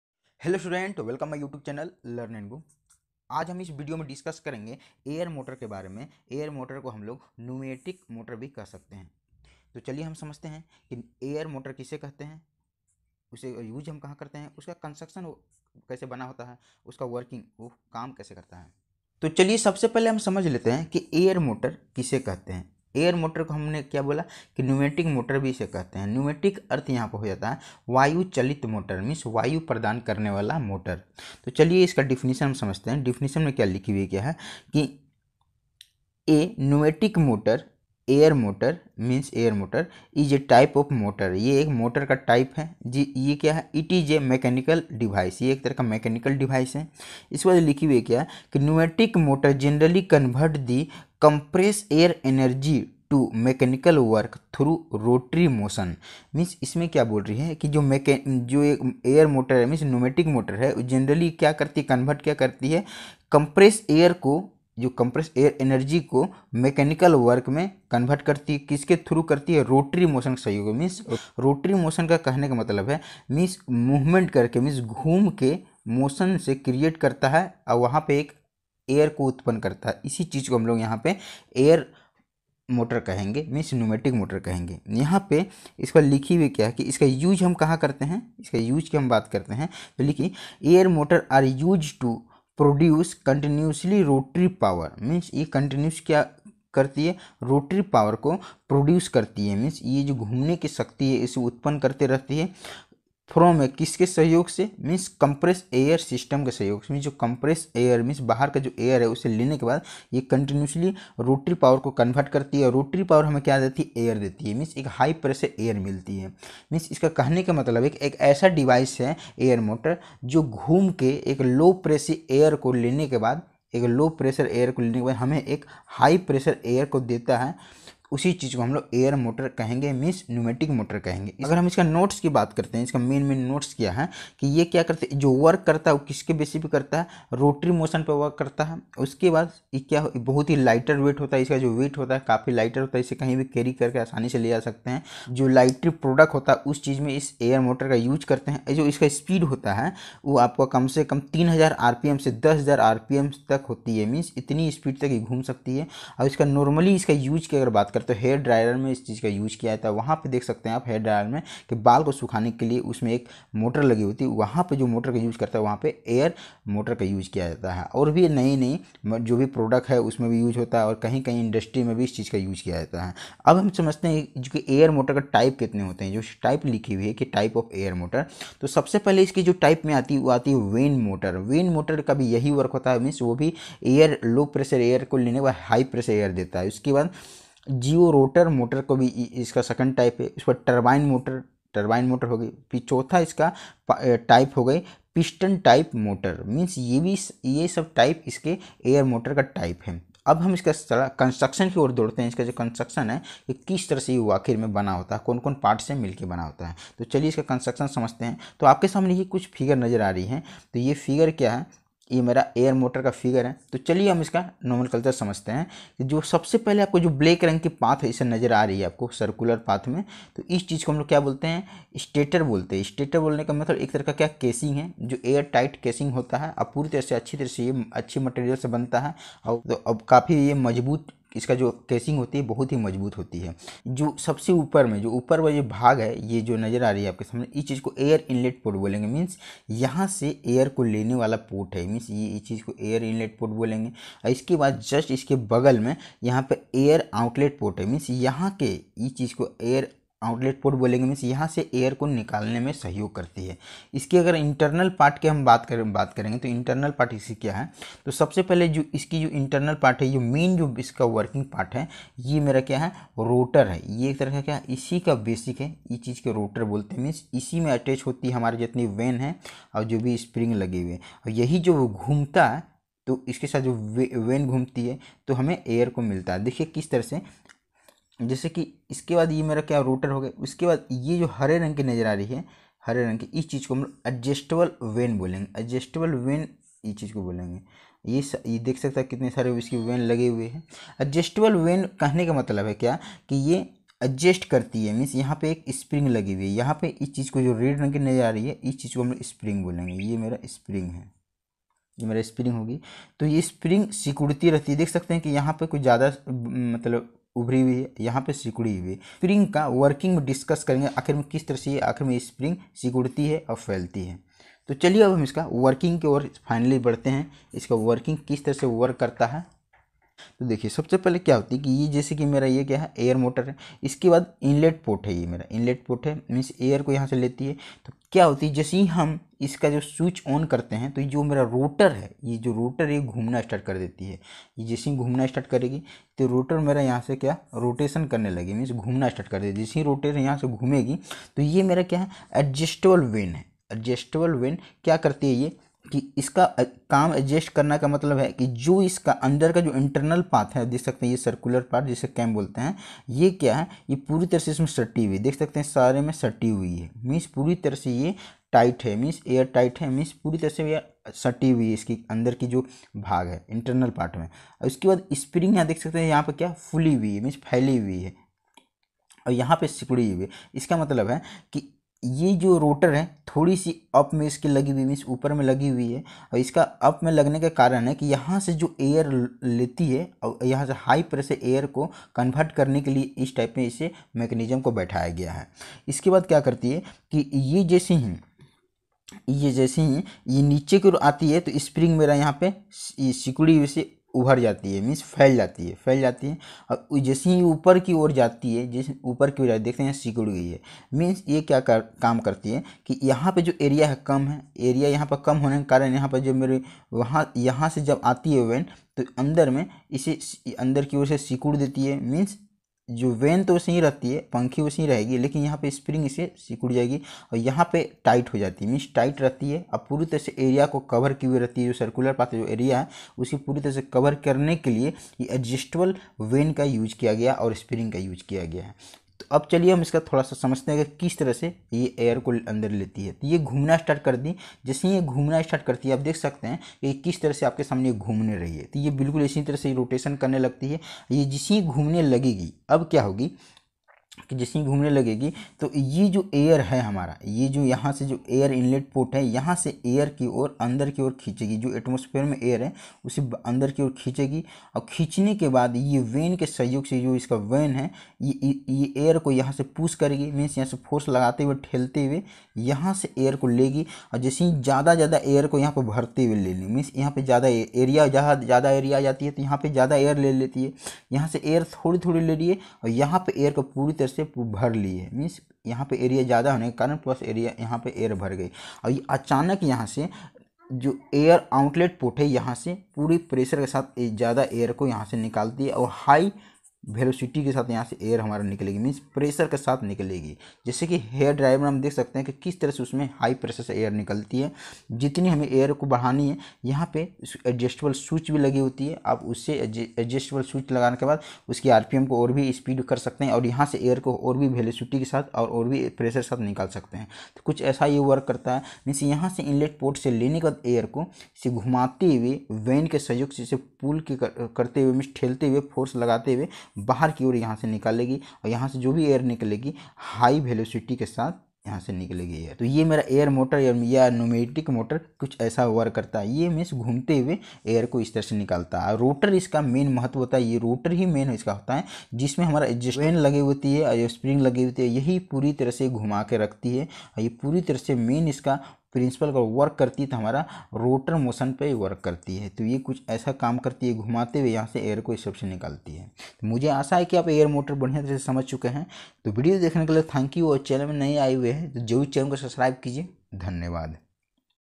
AIR MOTORS INTRODUCTION
Air motors are used to produce continuous rotary power from a compressed air system. ... They boast a number of advantages over electric motors: • Because they do not require electrical power, air motors can be used in volatile atmospheres.
Rotary pneumatic motors work when the energy from the compressed air is stored up and then routed to the motor chamber, which is sealed. The pressure gets blown out, and makes the rotor turn quickly. When the rotors begin to turn quickly, the motion is similar to that of a windmill